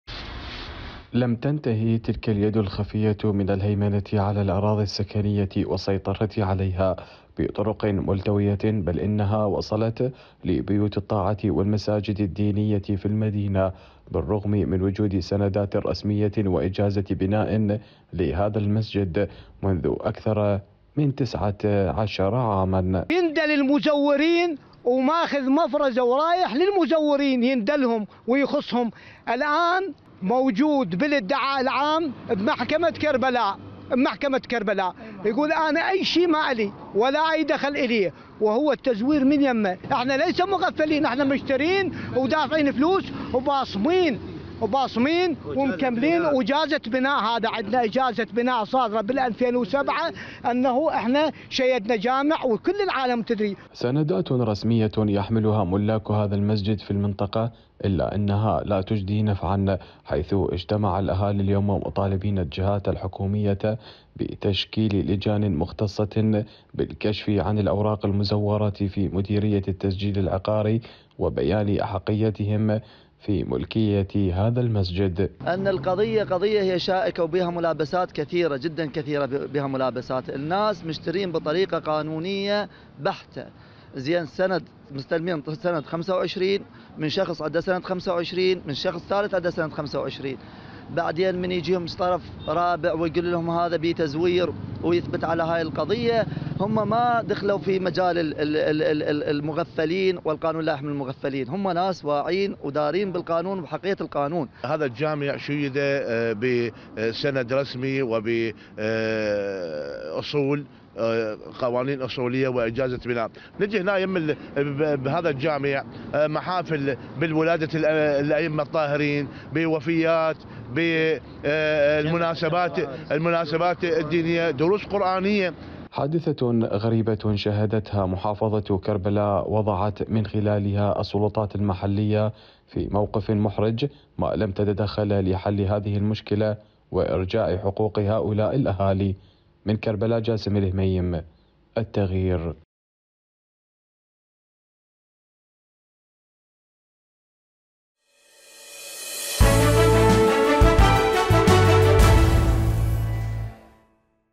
عصابة خطيرة تتحرك لسرقة وبيع جامع في كربلاء !! | تقرير
كربلاء .. حيث تحاول اياد خفية متنفذة الاستيلاء على جامع اهلي مشيد منذ نحو عقدين رغم وجود الأوليات الرسمية ضمن التسجيل العقاري والتي تشمل السند الرسمي واجازة البناء فيما يطالب الأهالي بالتحقيق والتدقيق في عمليات التزوير للاستيلاء على الجامع.

موقع قناة التغيير الفضائية  الالكتروني